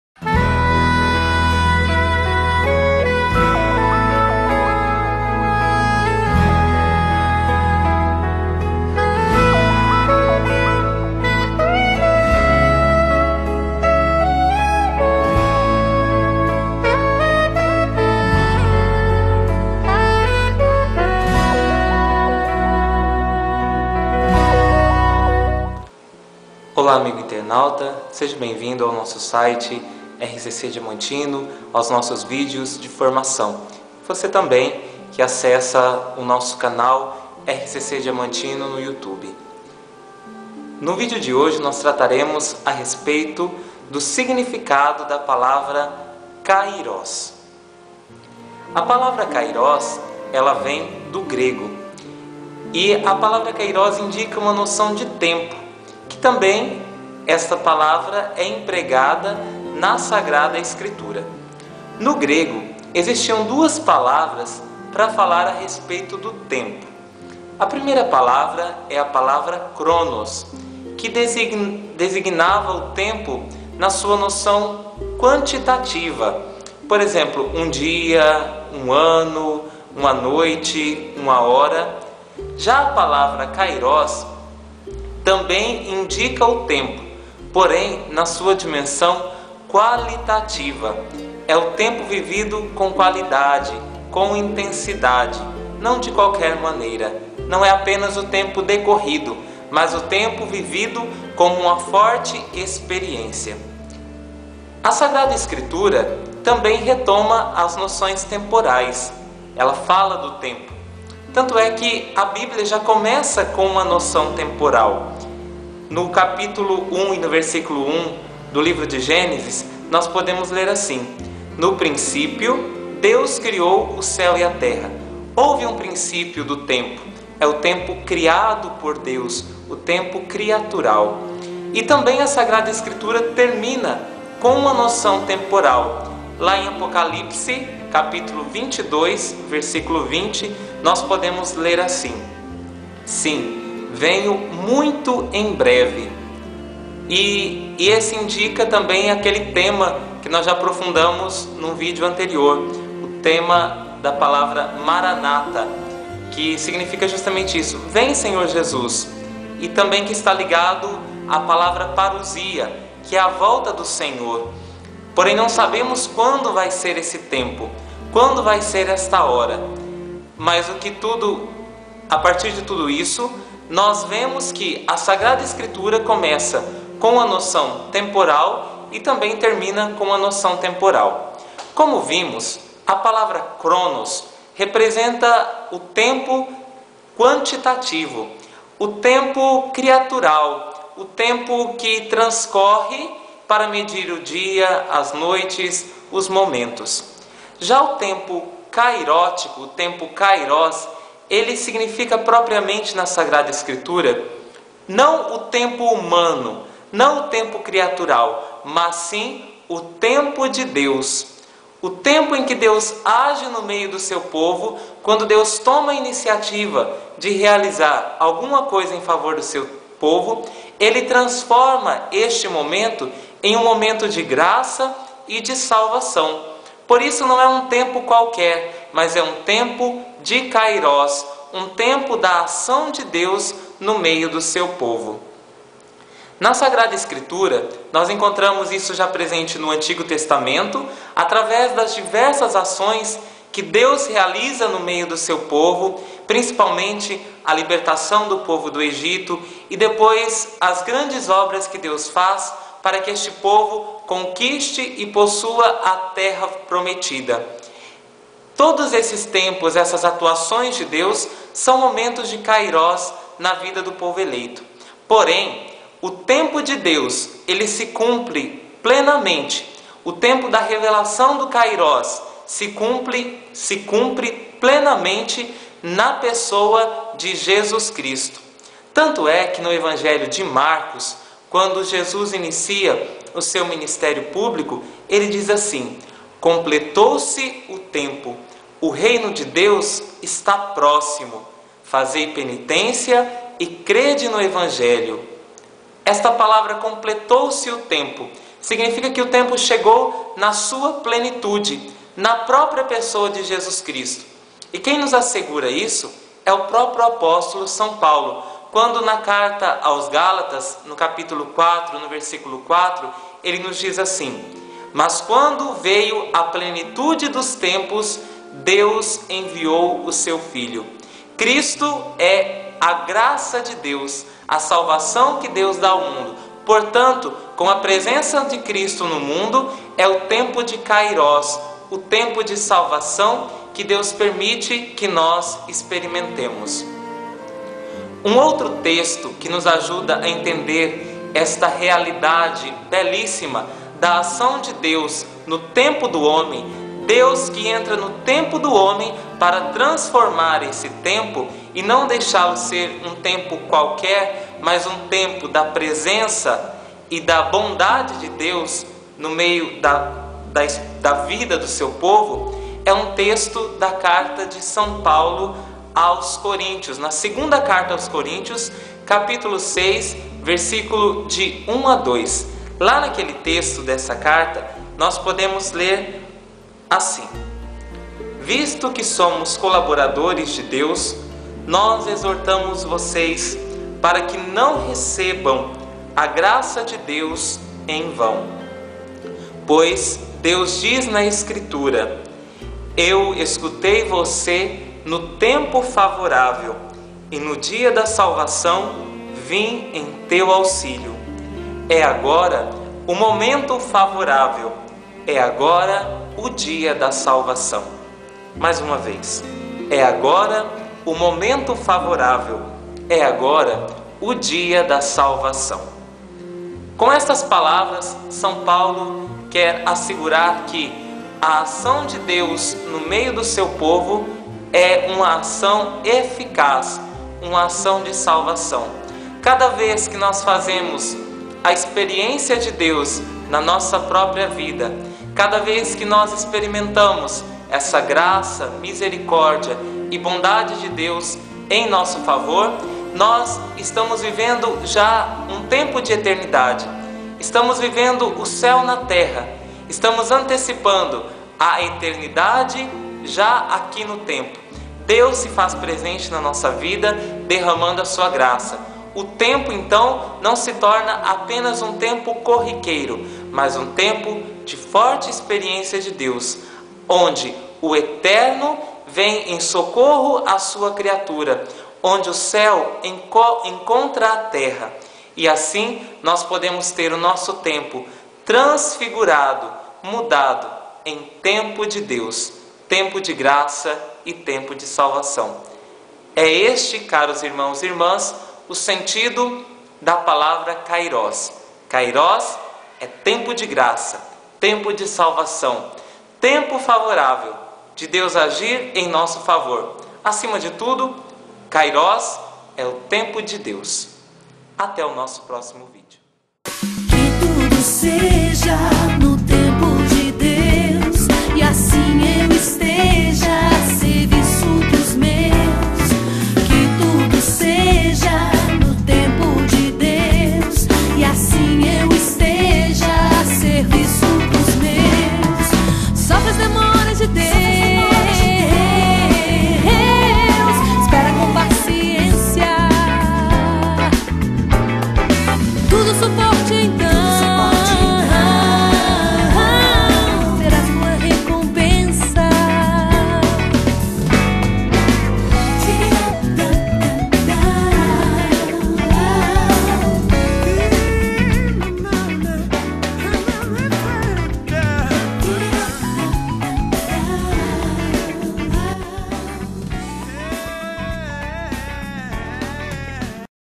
Kairós.wmv
Formação sobre o significado da palavra grega "kairós".